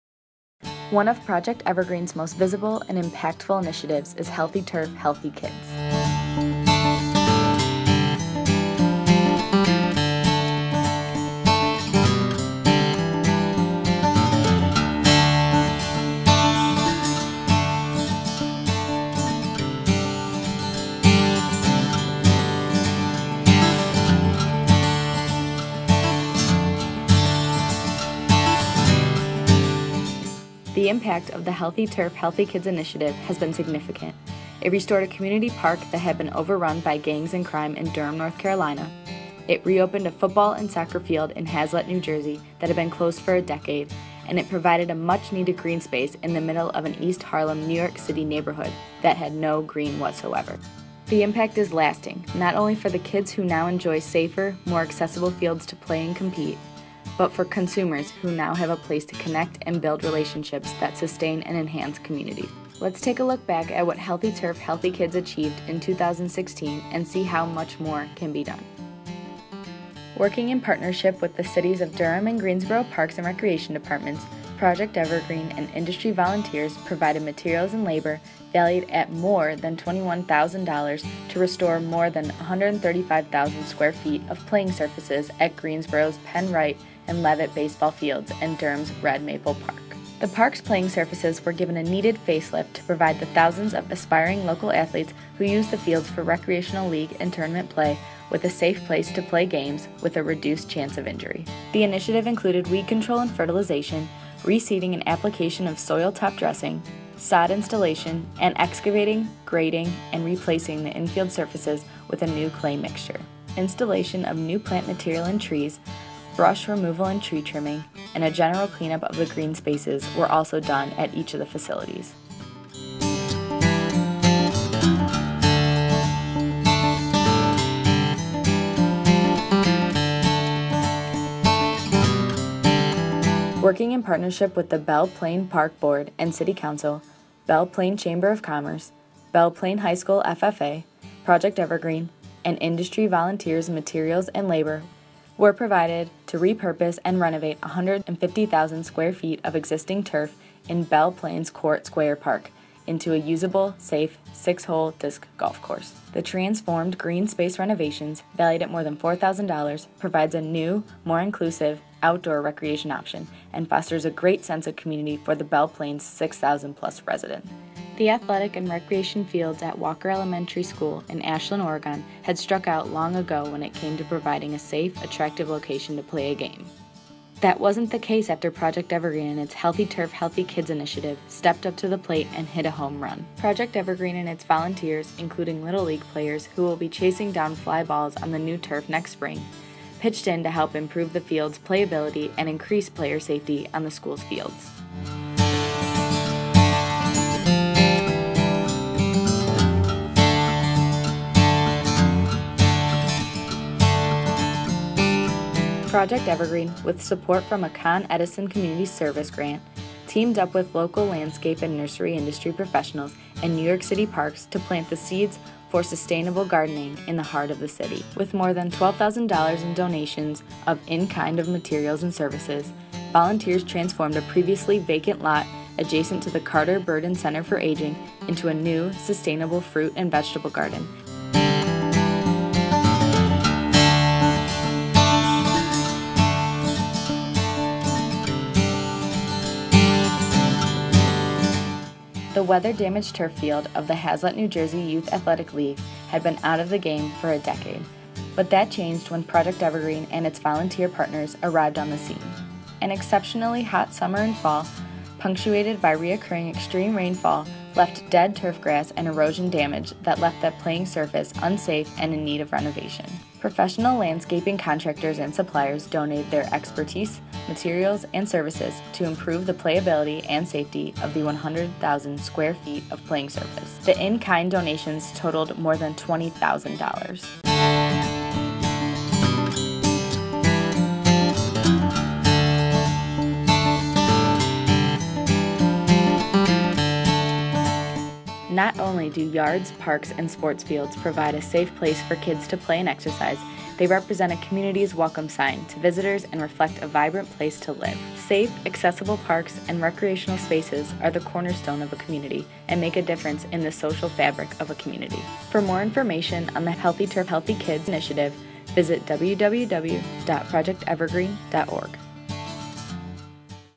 Project EverGreen 2016 Year In Review
2016 was a busy year for Project EverGreen. See what projects were completed and the impact it had on the communities.